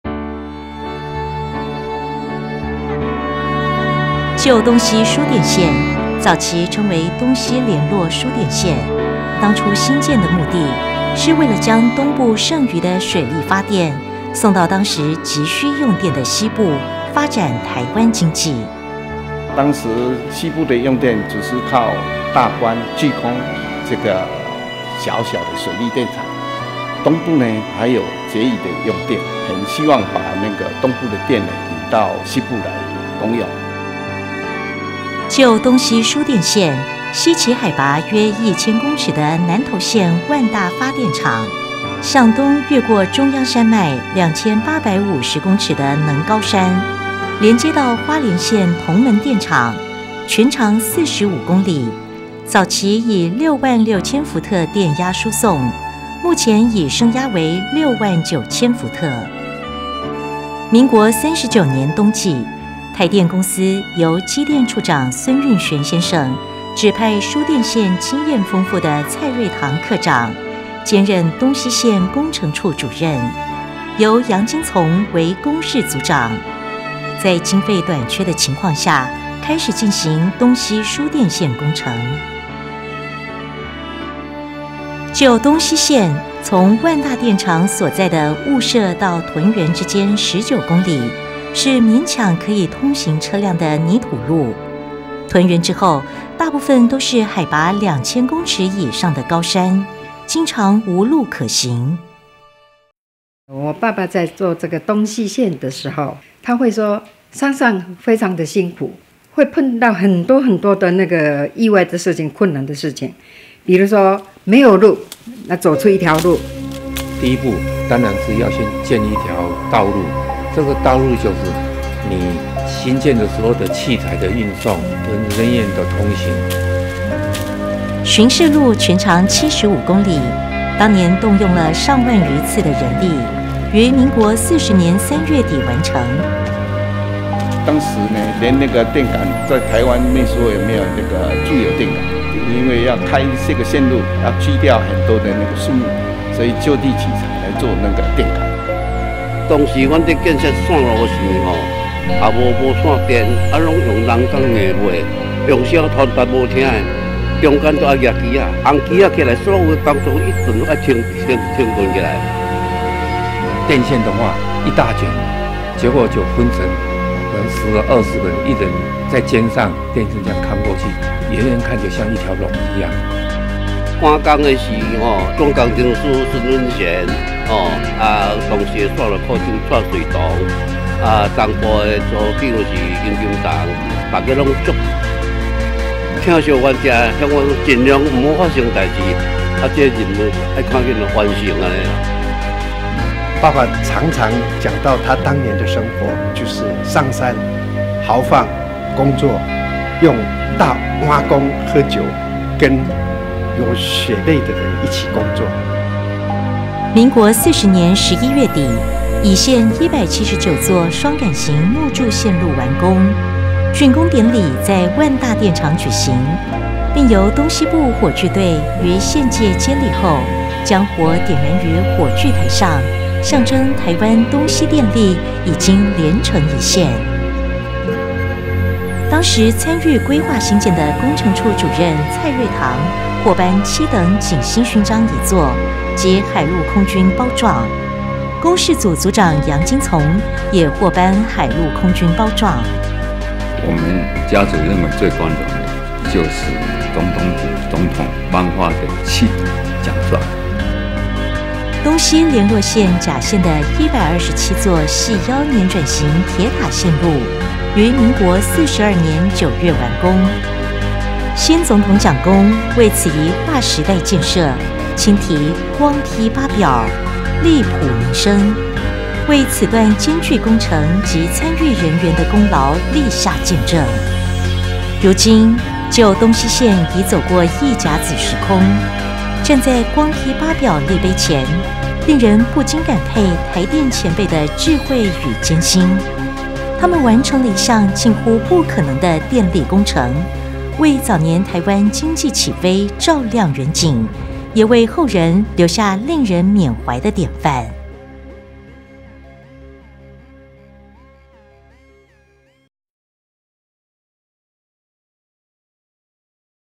台電舊東西線的故事
感謝網友提醒
影片2:32 出現前輩為張國安先生(誤植為陳)
電力是現代文明與經濟發展的原動力，台灣電力公司自台灣光復以來，本著篳路藍縷，以啟山林的精神，為國家建設奠定基礎，並見證了台灣經濟發展的奇蹟。
台灣光復初期，西部地區供電不足，台電公司克服了人力、物資與財力的不足，於民國40年及42年分別完成橫貫中央山脈海拔3000多公尺地區之特高壓電力輸電線路甲、乙兩回線，將東部多餘的電力輸往西部，此段輸電線路即為台電東西連絡線。
東西連絡線西起南投縣萬大發電廠，當地海拔約1,000公尺，越過海拔近2,850公尺之能高山，東至花蓮縣銅門發電廠，全長45公里，以66kV電壓輸送，所經之地，從霧社到屯原之間19公里，勉強可通卡車；從屯原到銅門，沿途峭壁危崖，山勢高聳，所經高山大都無路可行，想要通行難如登天。
架設輸電線前需先修建「保線路」作為人員通行、器材搬運的通道。保線路全長75公里，當年發動了上萬餘人的力量， 於民國40年3月底完成。這條專供搬運工程材料、器材及員工糧食補給之用的步道，是東西連絡線不可缺少的交通動脈。
工程人員興建電塔、架設電線，常需與大自然博鬥，高山地區每年12月至次年3月間經常積雪，且空氣稀薄，工程進行非常艱難，工作人員需餐風露宿，前仆後繼晝夜趕工，努力建設。
第一線路的179座雙桿型木柱線路，平均跨距217公尺，於民國39年興工，民國40年11月底完工；隨後進行第二線路的127座細腰轉型鐵塔及線路，平均跨距340公尺，最大跨距長達1322公尺，標高差達到2586公尺，是當時遠東海拔最高的輸電線，此線於民國42年9月完工。至此台灣東西電力系統合而為一，形成一個供電穩定的完整電力網，對台灣經濟發展有巨大的貢獻。